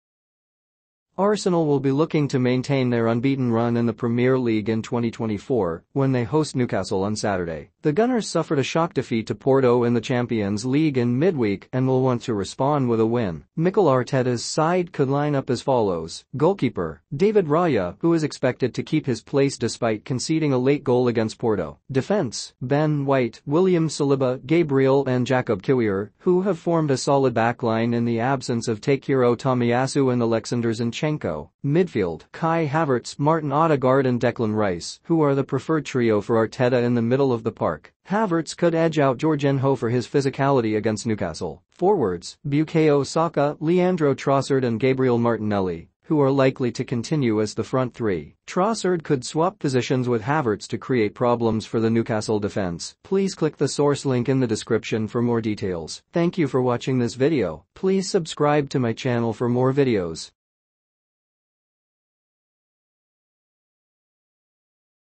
Arsenal team news: Predicted XI vs Newcastle as Mikel Arteta searches for Porto response
Arsenal will be looking to maintain their unbeaten run in the Premier League in 2024 when they host Newcastle on Saturday. The Gunners suffered a shock defeat to Porto in the Champions League in midweek and will want to respond with a win.
Mikel Arteta’s side could line up as follows:
Goalkeeper: David Raya, who is expected to keep his place despite conceding a late goal against Porto.
Defence: Ben White, William Saliba, Gabriel and Jakub Kiwior, who have formed a solid backline in the absence of Takehiro Tomiyasu and Oleksandr Zinchenko.
Midfield: Kai Havertz, Martin Odegaard and Declan Rice, who are the preferred trio for Arteta in the middle of the park. Havertz could edge out Jorginho for his physicality against Newcastle.
Forwards: Bukayo Saka, Leandro Trossard and Gabriel Martinelli, who are likely to continue as the front three. Trossard could swap positions with Havertz to create problems for the Newcastle defence.